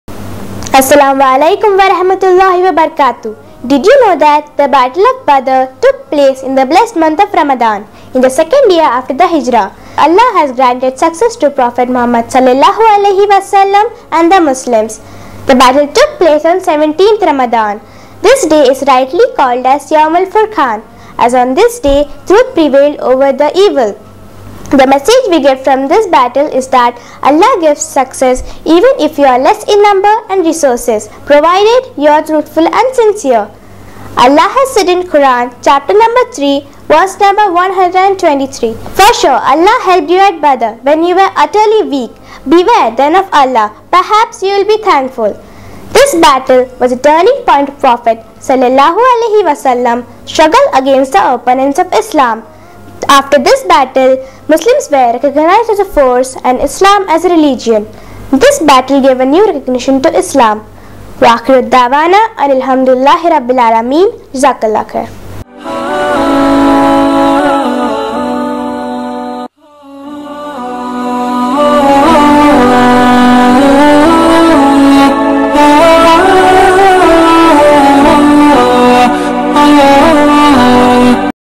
RAMADAN DID YOU KNOW SERIES - Day 17 - Battle of Badr took place in Ramadan
Ramadan short videos by IMA Kuwait